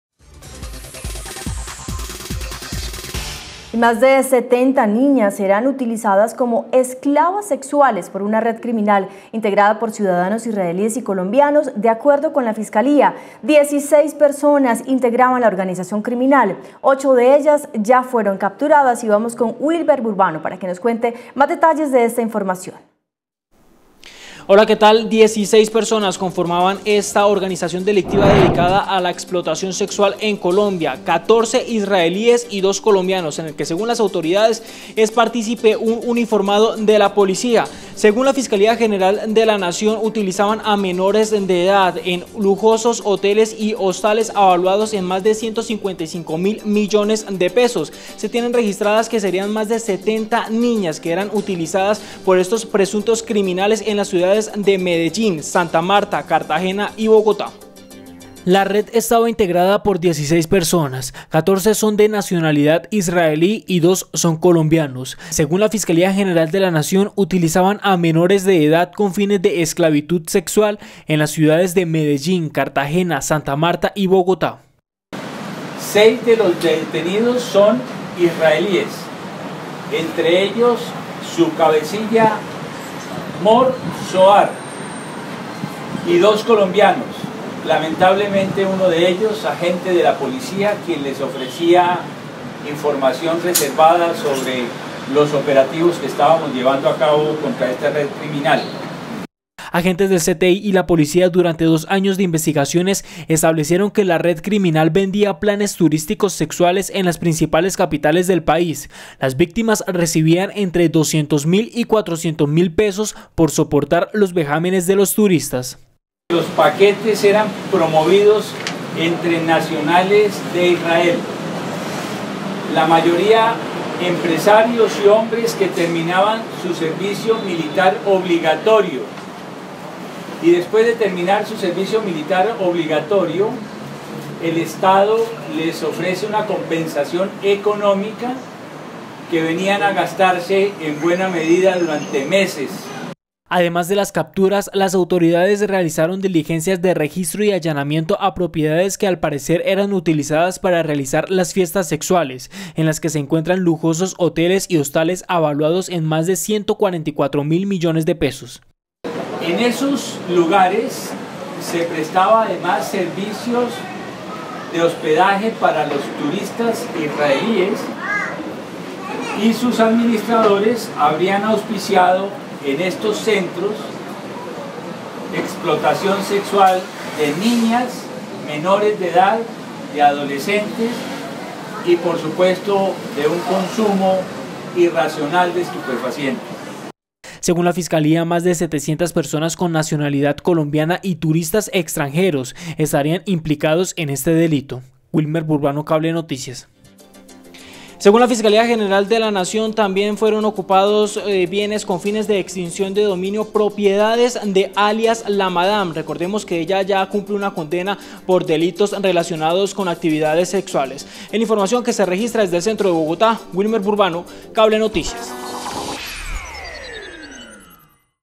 Golpe en Colombia a una poderosa red de explotación sexual infantil
Las autoridades detuvieron en varias ciudades del país a seis israelíes y dos colombianos que hacen parte de una red de explotación sexual de menores de edad con tentáculos en Centro y Suramérica, informó la Fiscalía General de la Nación.